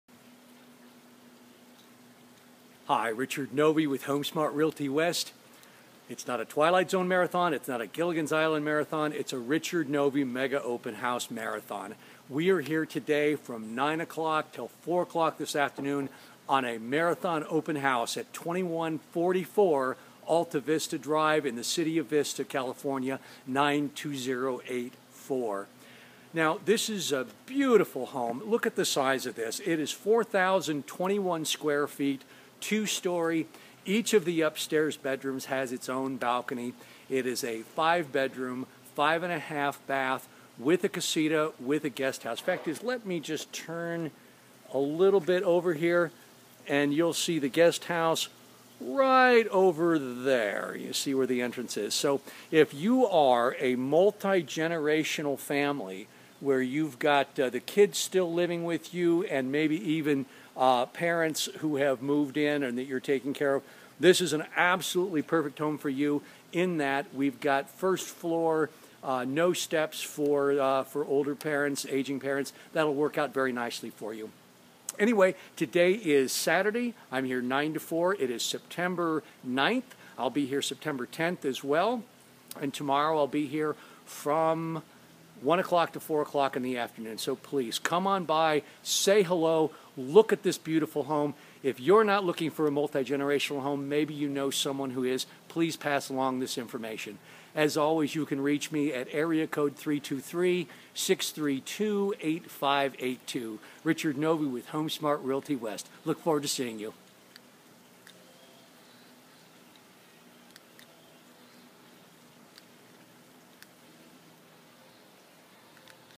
Another Mega Open House from Richard Novey
Designed by Architect Mark Singer! Rare opportunity to own one Vista's premiere luxury properties, Highly upgraded custom home is conveniently close to all amenities of life and has been maintained and cared for over the years from the beautiful landscape to the spacious interior to the generous rooms with the recently updated kitchens and living areas. Offering a 1 bedroom GUEST HOUSE, A bright and open floor plan featuring cathedral ceilings, with a pool a view and much more.